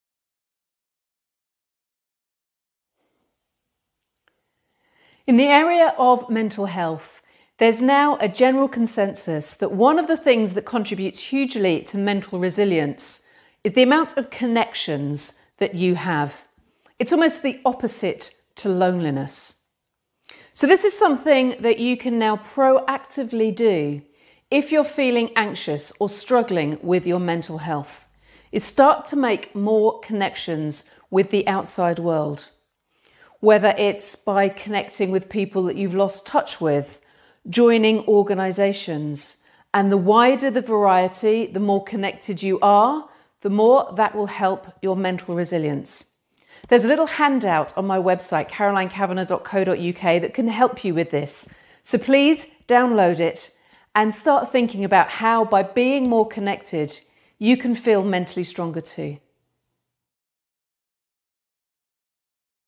How to reduce anxiety by building connections
Research shows that the more people we are connected to, the more mentally resilient and less anxious we tend to be.  It makes sense, but where do you start to extend your network of contacts, how do you connect with people?

There is an e-book that you can use to help you take the next steps for you to build more connections.  Download that here

For more downloadable resources and advice on reducing anxiety, please go